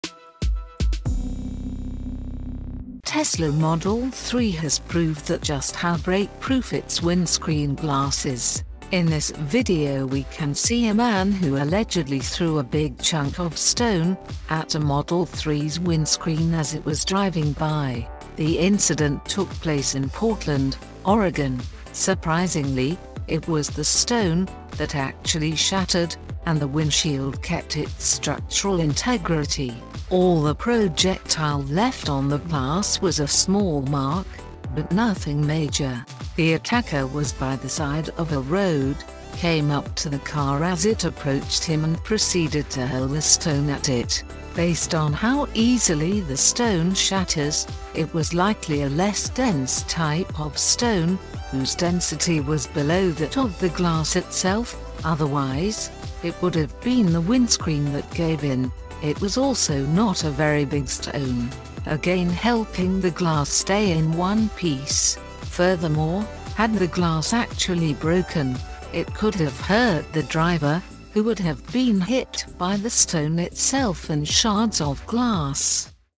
Tesla Model 3 Windscreen Deflects And Shatters A Massive Rock
Tesla Model 3 has  proved that  just how break-proof its windscreen glass is.

 In this video we can see a man who allegedly  threw a big chunk of stone, at a Model 3’s windscreen as it was driving by.